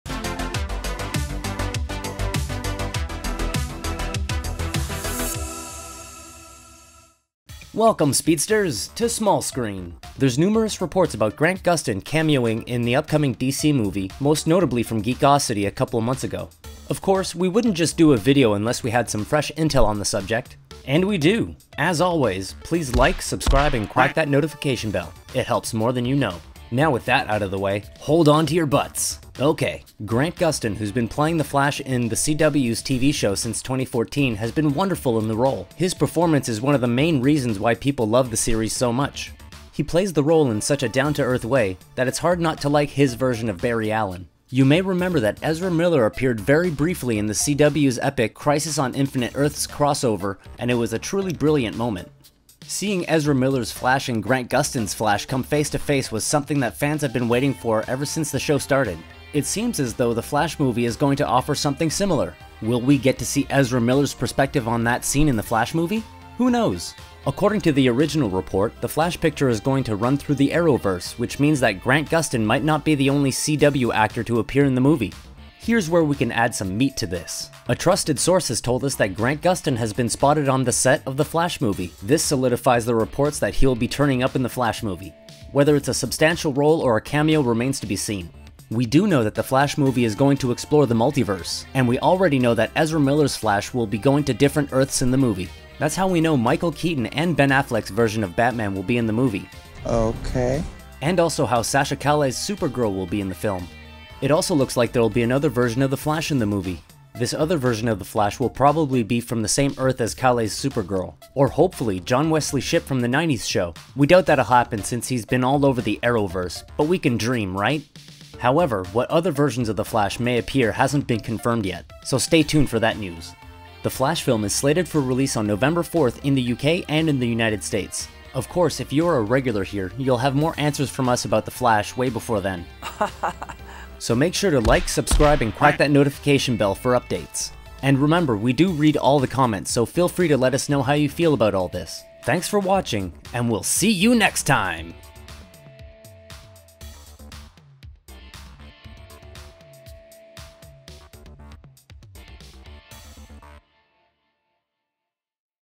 Grant Gustin To Cameo In Ezra Miller's The Flash Movie
Sources close to Lucasfilm have informed us that Lars Mikkelsen has signed on the dotted line to star as Grand Admiral Thrawn in the upcoming Ahsoka spinoff show.

Written by That James Guy & Daniel Plum based on articles by Edward  Lauder

Produced By That James Guy

Edited By Dan Plum

Narrated by Edward Lauder

3D Animation by Kaz.